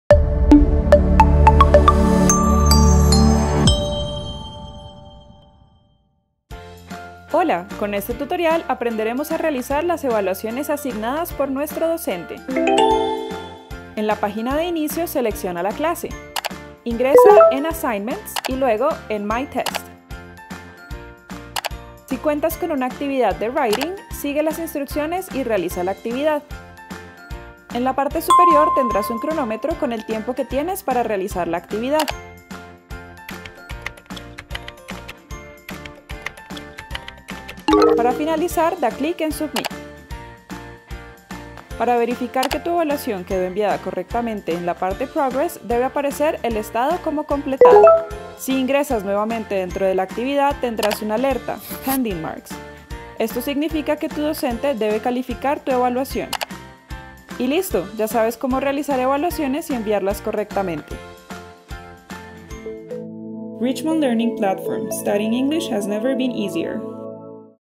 REALIZAR EVALUACIONES-ESTUDIANTES
¡Hola!, con este tutorial aprenderás como realizar las evaluaciones asignadas por tu docente